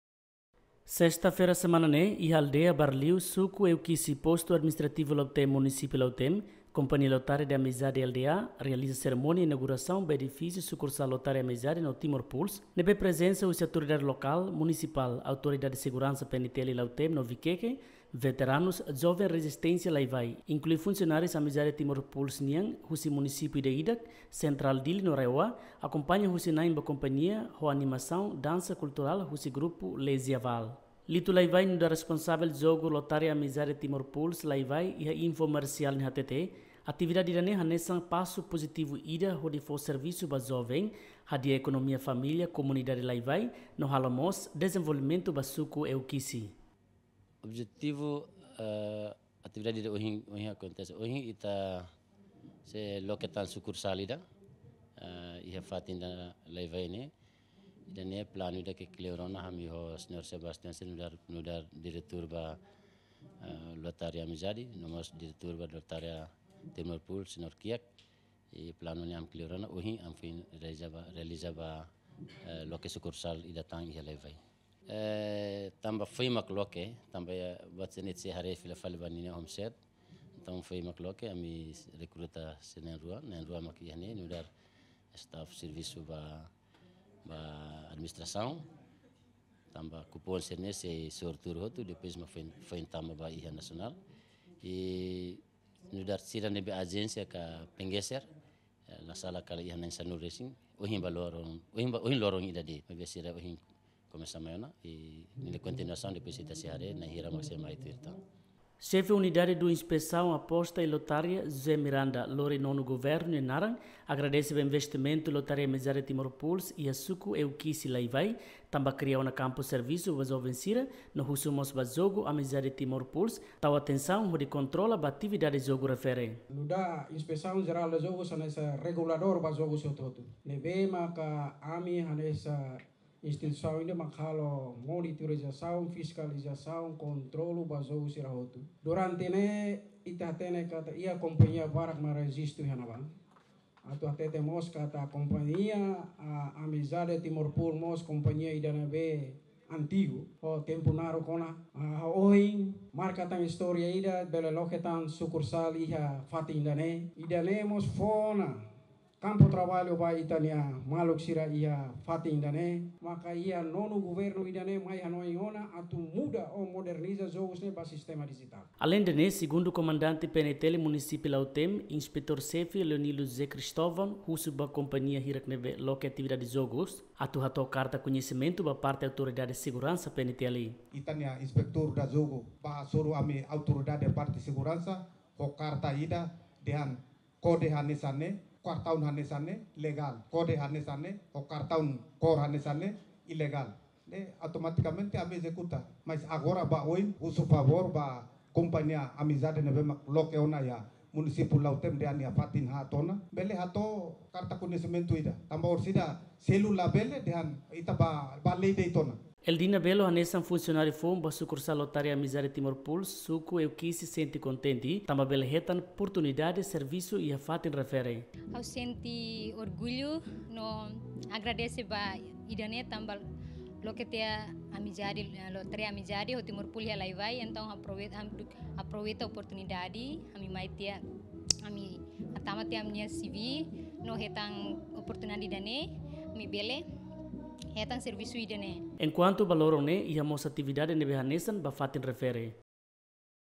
Lotária de Amizade Lda hala’o Inaugurasaun edifísiu Sukursal iha suku Eukisi Laivai Munisípiu Lautem
Lotária de Amizade Lda hala’o Inaugurasaun ba edifísiu Sukursal iha suku Eukisi Laivai Munisípiu Lautem